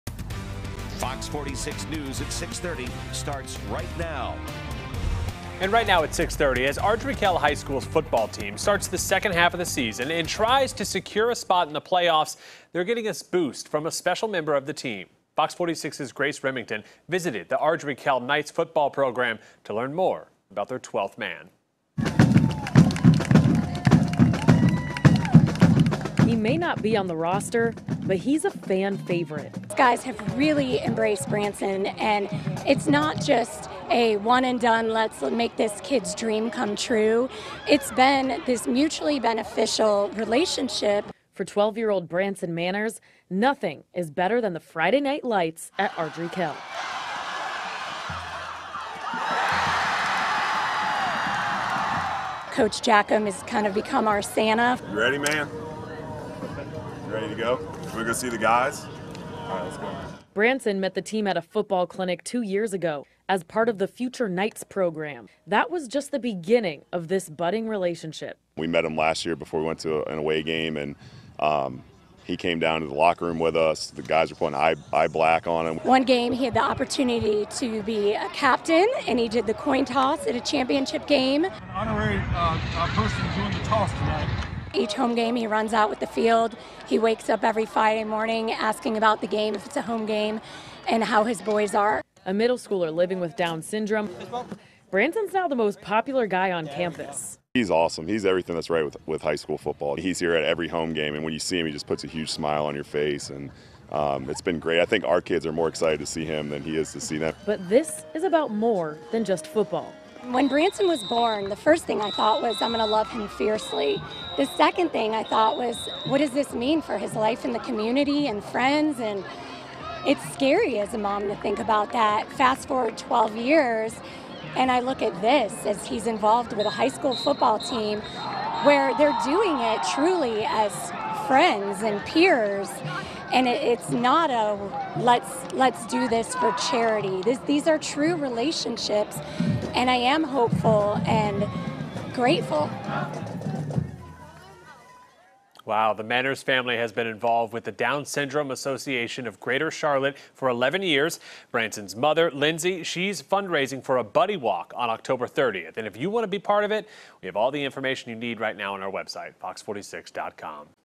Ardrey Kell High School football's 12th man
The Ardrey Kell High School football team is getting a boost from a special member of the team.